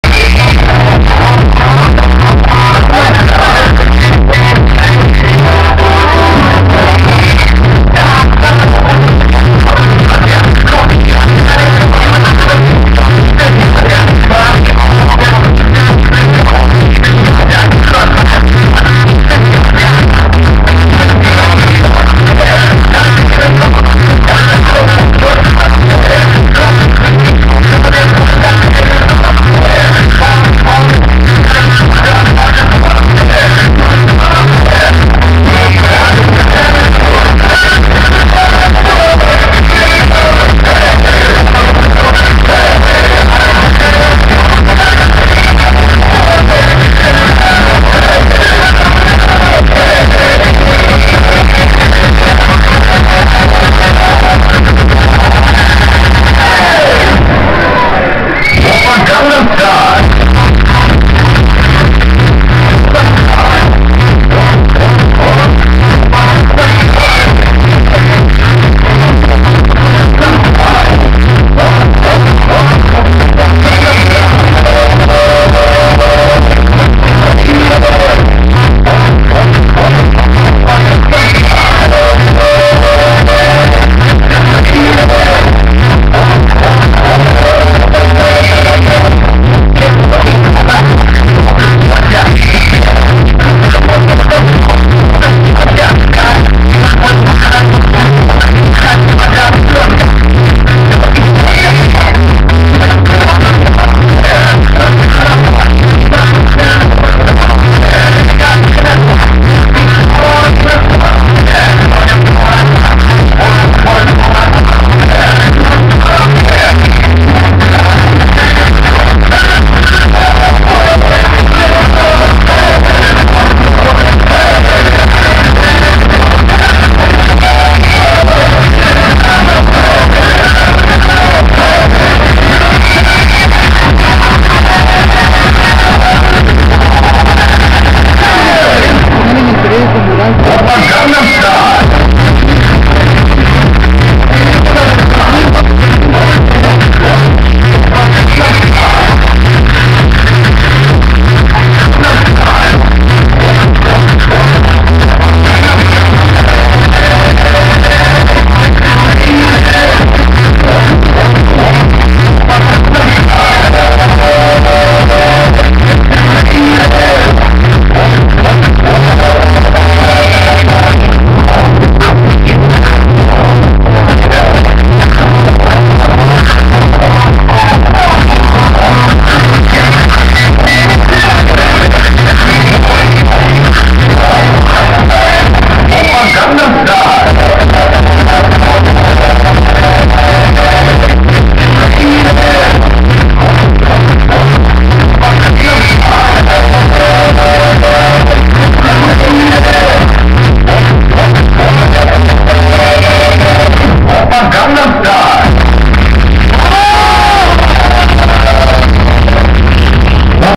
Baile del caballo
Baile en la escuela, Pablo Neruda,,dia del Maestro participan, los niños de 6ª"A"